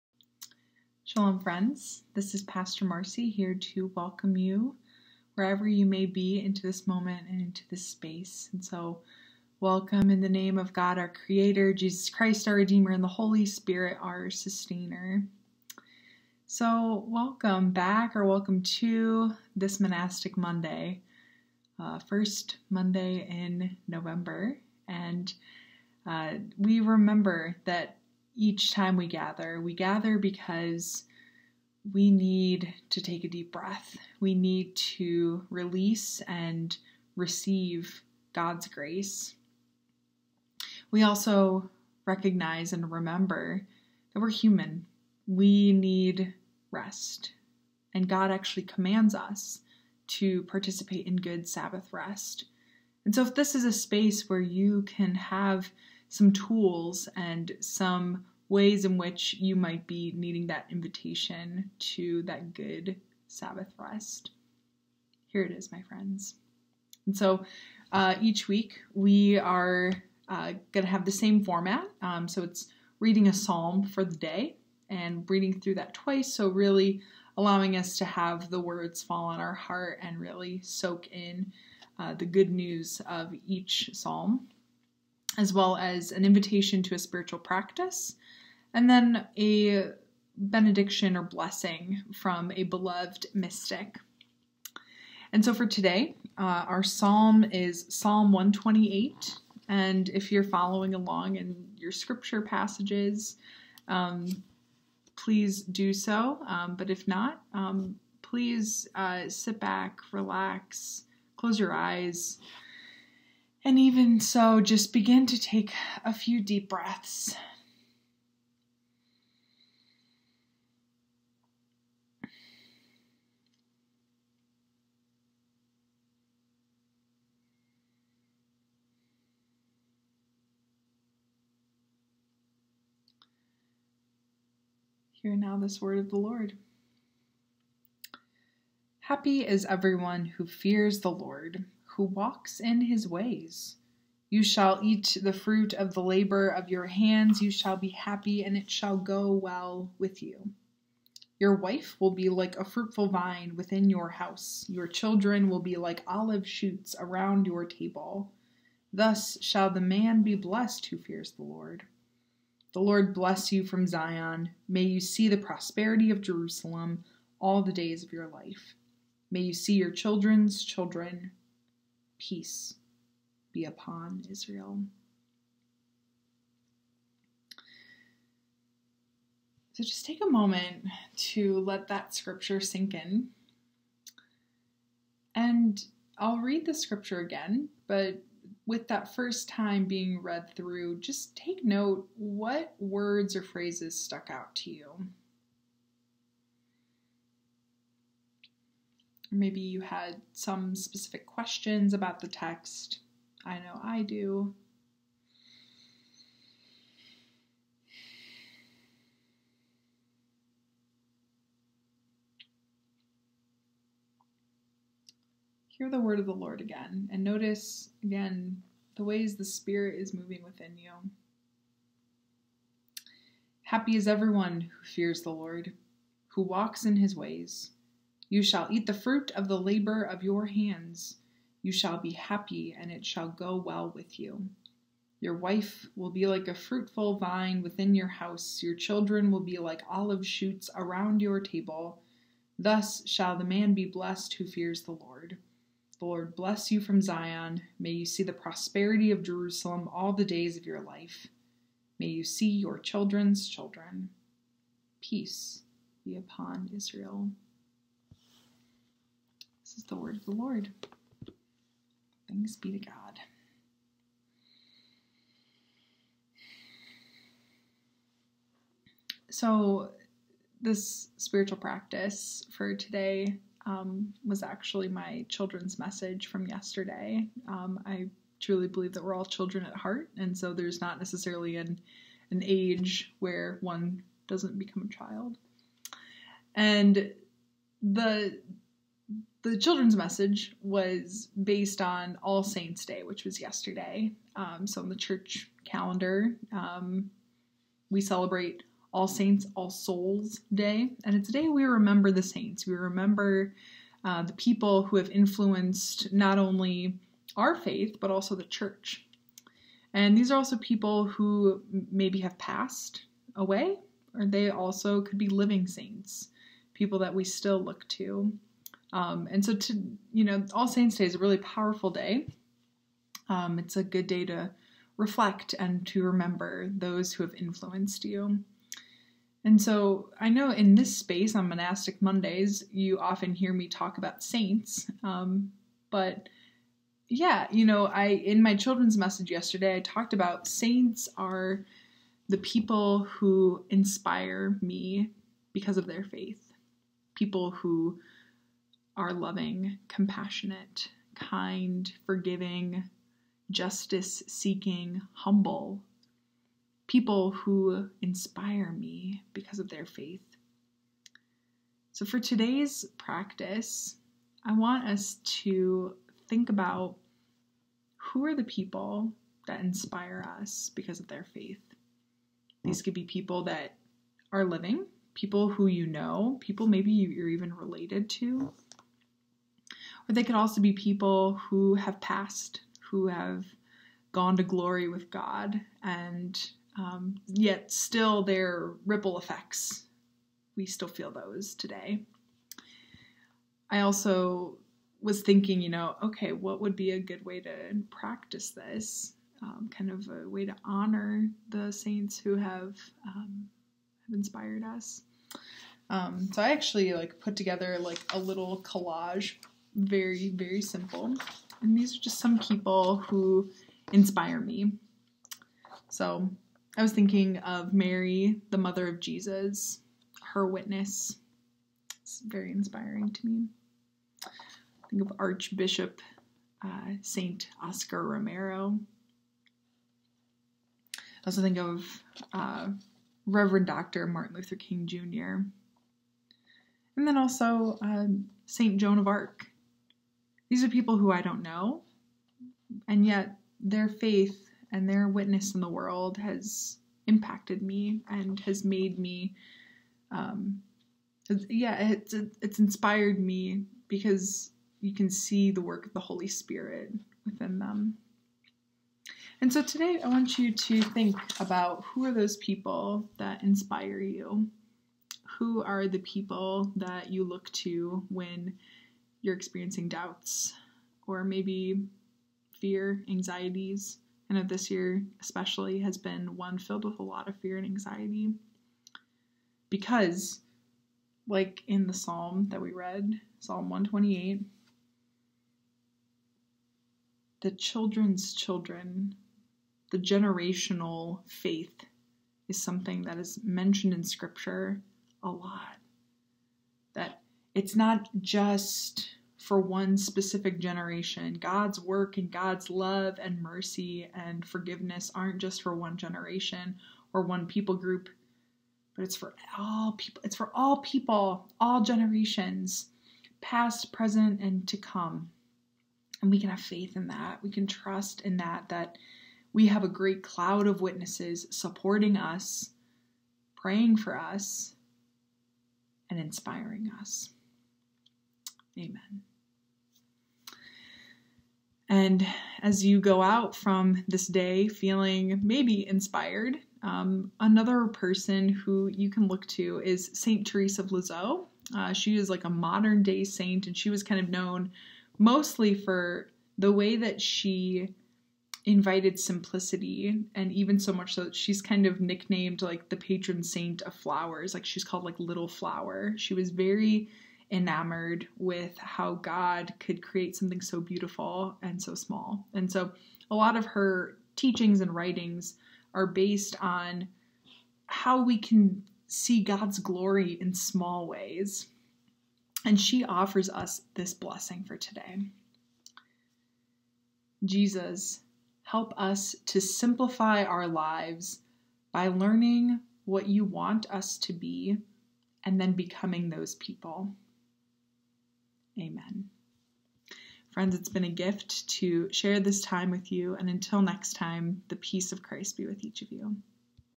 Monastic Mondays with Pastor Marcy- November 2nd, 2020
Grace and peace friends,
Each Monday I would like to offer us some space set apart for us to reflect, meditate, and connect with one another. I'll provide a Psalm, a Meditation or some sort of spiritual practice, and a Blessing from a beloved mystic.

May this time be one filled with compassion for yourself and unexplainable peace.

Peace be with each of you.

Be held,
Pastor Marcy